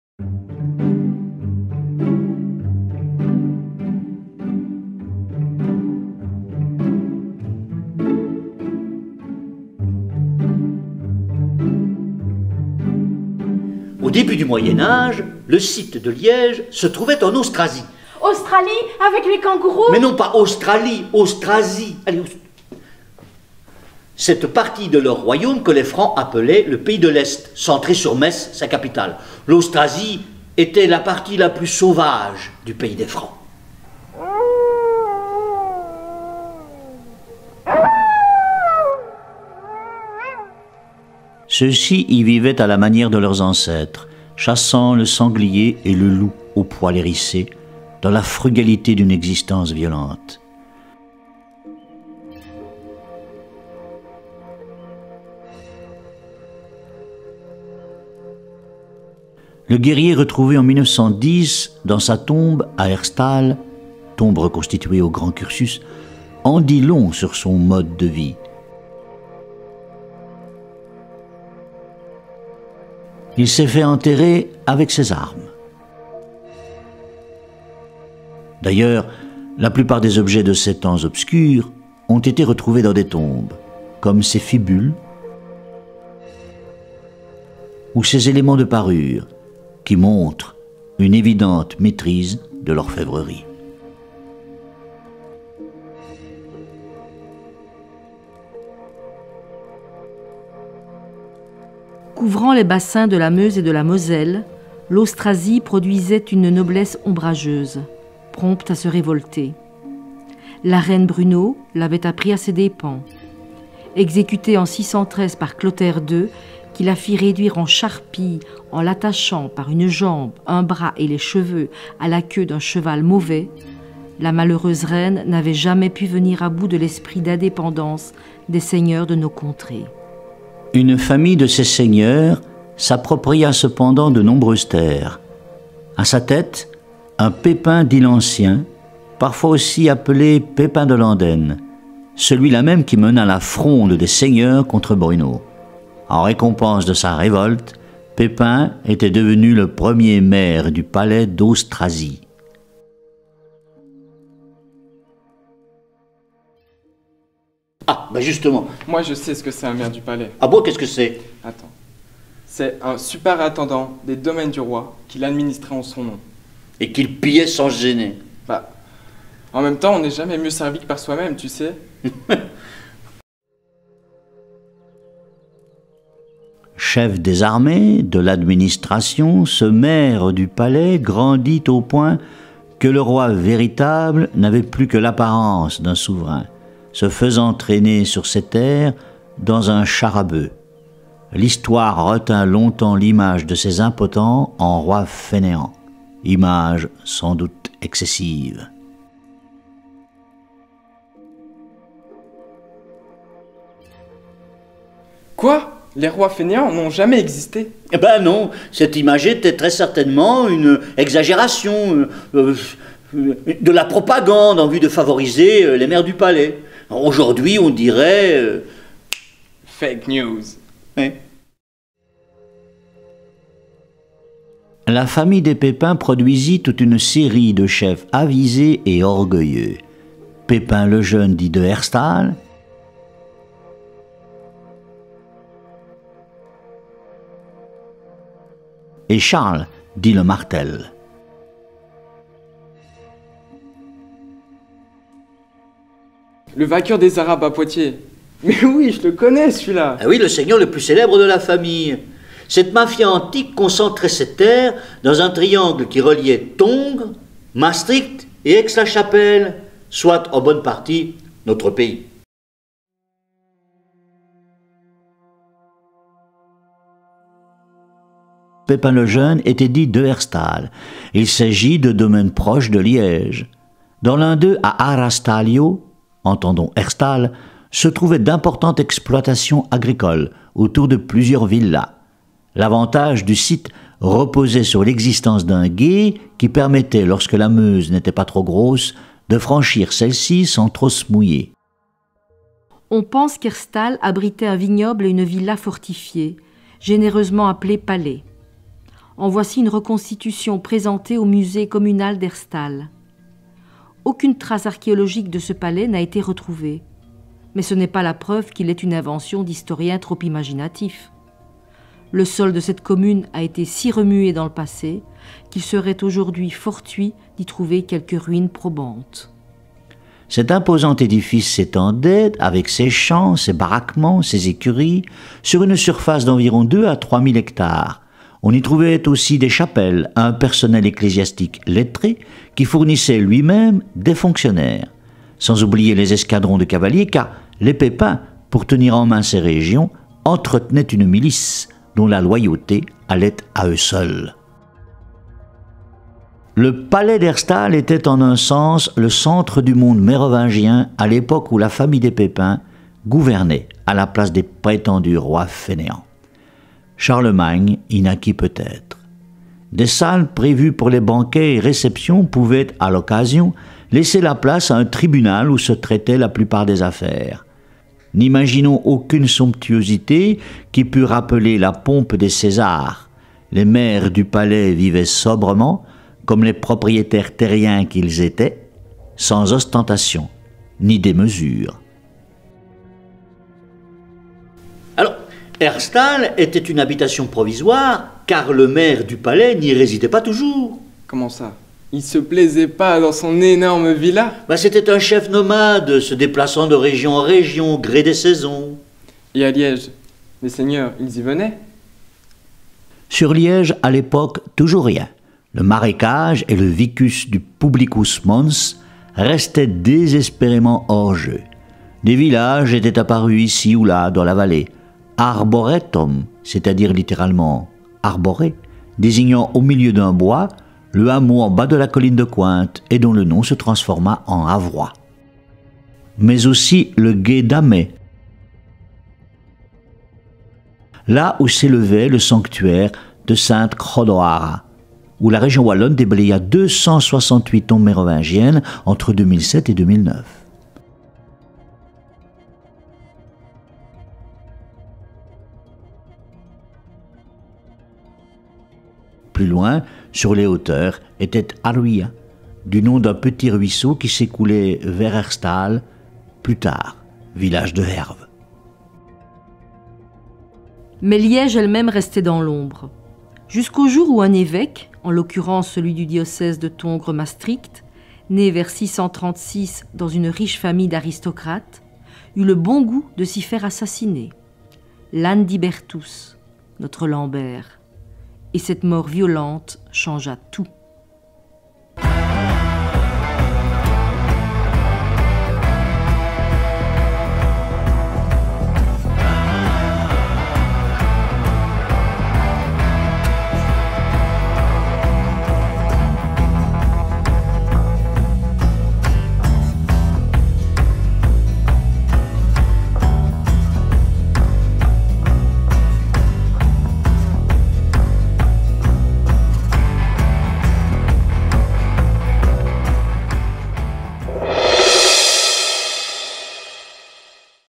Liège au temps des Francs / Liège ma Ville - 4
Quelle est l'histoire de Liège au temps des Francs ? Balade autour du passé et des principaux monuments de Liège, cité millénaire. Pour amateurs d'histoire et amoureux du terroir...